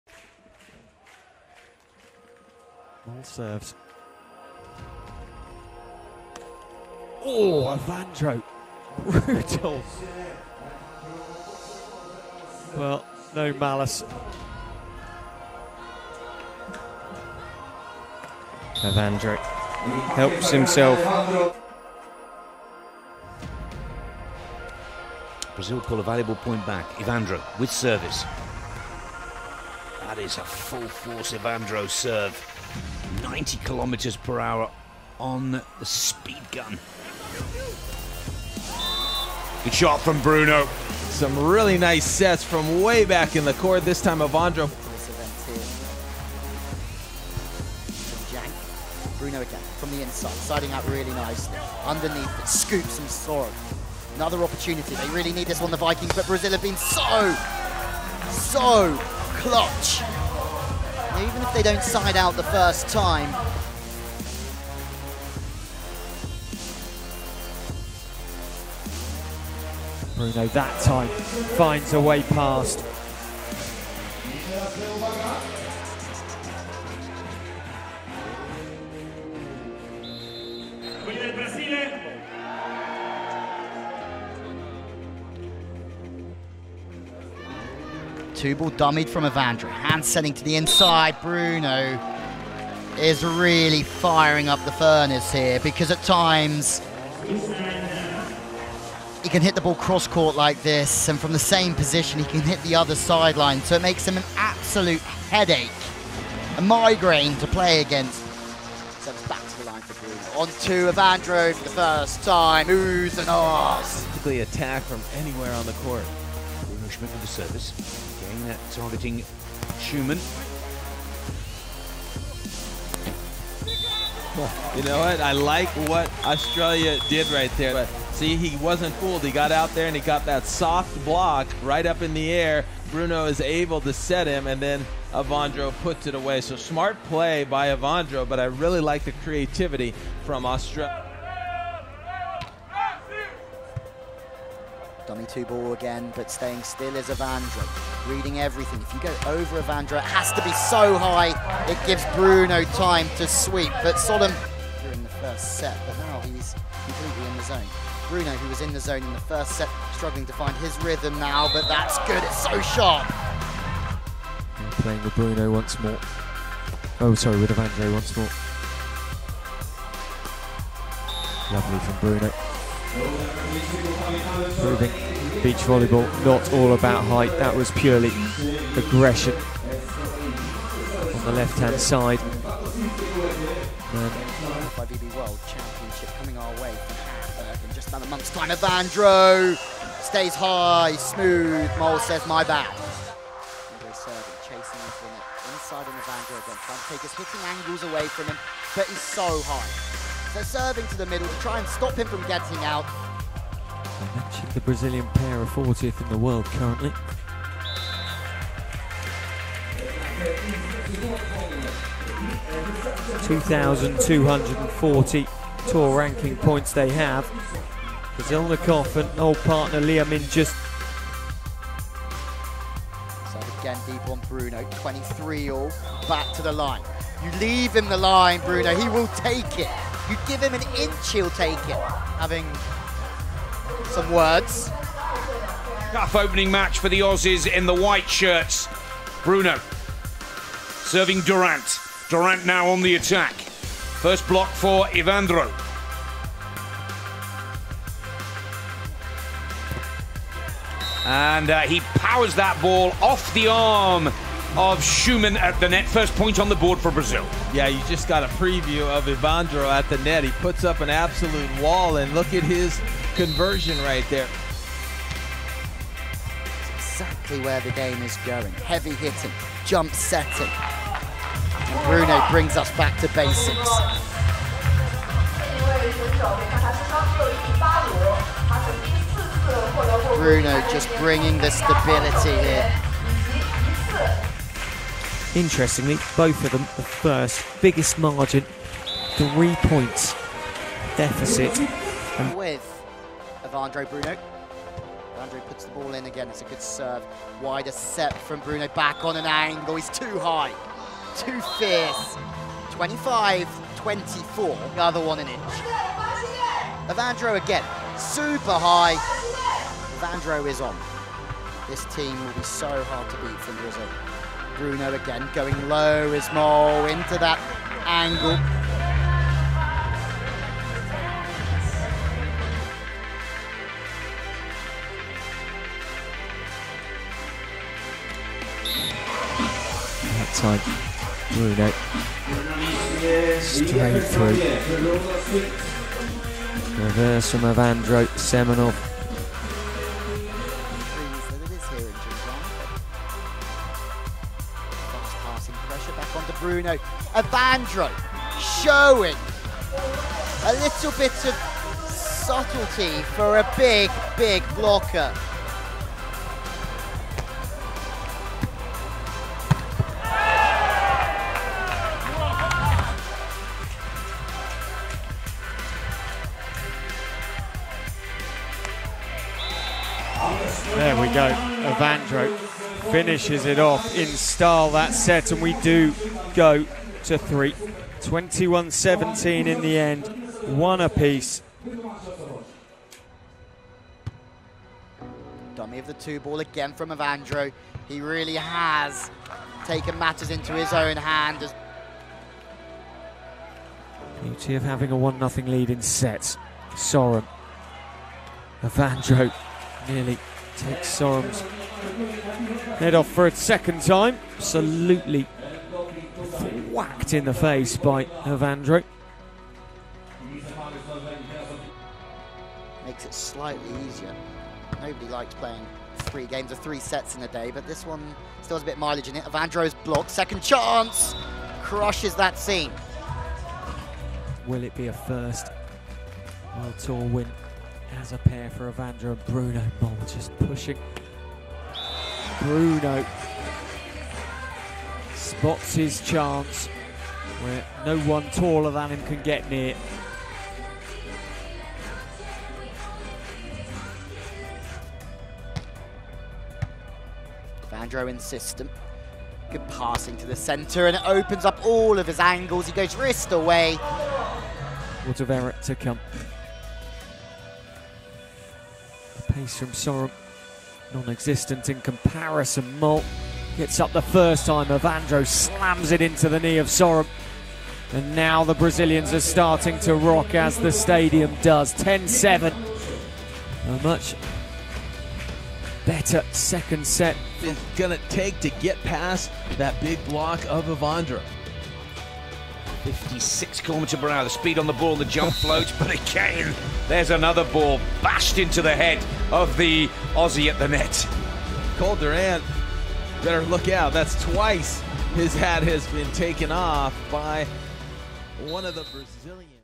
MOST BRUTAL SPIKES by Evandro/Bruno Schmidt! | Highlights Beach Volleyball World
The Brazilian elite Beach Volleyball team Evandro/Bruno has decided to join forces in February 2019. But results do not always match expectations immediately. The 17th-place finish they had on their debut at the 4-Star tournament in Doha (Qatar) repeated a month later in Xiamen (China) and then things started to get better for the team. The 33-year-old Bruno and the 29-year-old Evandro started to show glimpses of the team they could be when they took a silver medal in Jinjiang (China) at the end of May. Less than a month later, they finally fulfilled their potential by topping the podium in another 4-Star tournament, this one in Warsaw (Poland) where they defeated world number ones Anders Mol and Christian Sørum of Norway in the Final.

With uniting Evandro’s phenomenal serving ability and Bruno’s impressive backcourt skills, this very promising team has everything to play for Gold at the 2021 Olympics in Tokyo - Enjoy watching their best Spikes so far! FIVBTOTW Evandro/Bruno

LOVE VOLLEYBALL?